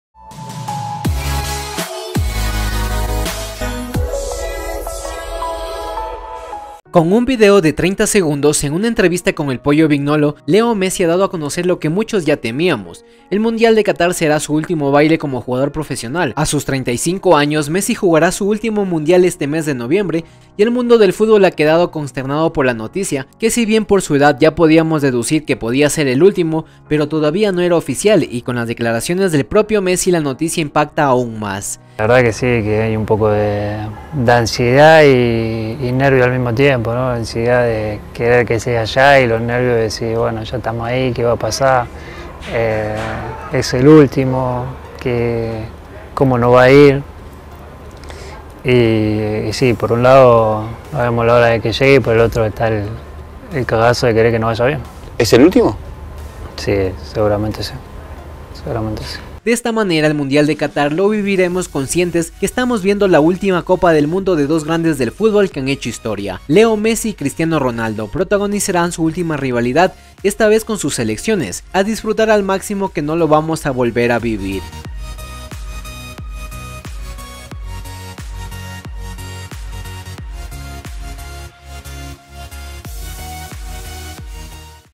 ¡El TRISTE ANUNCIO de MESSI! “SEGURAMENTE CATAR será mi ÚLTIMO MUNDIAL!
Apenas mes y medio antes del inicio de la Copa del Mundo de Qatar 2022, Lionel Messi, capitán y estrella de la Selección argentina ha ofrecido una entrevista para Star+, donde ha dejado una de las declaraciones más relevantes que se podían anunciar a nivel de selecciones: “La verdad es que hay un poco de ansiedad y de nervios al mismo tiempo. es el último, ¿cómo nos va a ir?’. Por un lado no vemos la hora de que llegue y por otro está el cagazo de querer que nos vaya bien”.

Ante la sorpresiva noticia, el entrevistador no pudo evitar la pregunta que confirmase que sus oídos habían oído correctamente: “¿Es el último?”. A lo que Messi respondió: “Sí, seguramente sí”.

Además, el atacante del PSG afirmó que “cuenta los días” para que llegue la cita de Qatar, donde ve a la Albiceleste como campeones, pero no los etiqueta como favoritos: “No sé si somos los grandes candidatos, pero creo que la Argentina de por sí es candidata siempre, por su historia, por lo que significa y más ahora en el momento en el que llegamos... Pero no somos los máximos favoritos, me parece a mí. Creo que hay otras selecciones que están por encima de nosotros, pero estamos ahí... muy cerquita”.

Por otra parte, Messi tuvo unas palabras acerca de la dificultad de un torneo que va a disputar por quinta vez, además de resaltar el buen momento de forma de los de Scaloni: “La verdad es que llegamos en un buen momento, por cómo se dieron todas las cosas, con un grupo muy armado y muy fuerte. Pero después en el Mundial puede pasar de todo. Todos los partidos son dificilísimos y por eso es tan difícil y tan especial, porque no siempre los favoritos son los que terminan ganando y haciendo el camino que uno esperaba”.

La alegría de la Copa América
En la entrevista, Messi también habló del último título conquistado por la Selección argentina, la Copa América 2020. El ex del Barça habló de las sensaciones al convertirse en campeón de aquel torneo: “No lo podía creer. No sé cómo explicar ese momento. Es como decir: ‘Ya está, se dio’. Era lo que me faltaba, uno de los objetivos que me faltaban para terminar de cerrar todo. Para mí era fundamental poder ganar algo con la selección. Si no, me iba a quedar siempre la espinita esa de las finales perdidas y de no poder consagrarme”, declaró.

Tras esto, Leo añadió: “Fue una locura hermosa. Nunca lo había disfrutado así y fue una locura cómo lo vivió el país y los días después. Hasta el día de hoy creo que la gente está loca con la selección e ilusionada con lo que se viene. Por eso está bueno también bajar un poco los decibeles y que no nos creamos que somos los campeones y que le vamos a pasar por encima al que venga”.

Bonitas palabras para Scaloni
Messi también dedicó una parte de la entrevista al que fuera su compañero en la Selección mayor, y es ahora el DT de la misma, Lionel Scaloni, al que el 7 veces Balón de Oro tiene en gran estima: “Es una persona muy cercana, habla muchísimo con el jugador, intenta estar siempre para lo que sea y vive el fútbol muchísimo. Trabaja por y para el fútbol, le encanta lo que hace, sufre muchísimo los partidos y se merece todo lo que vive porque no fue fácil lo que le tocó agarrar. Él armó todo esto”.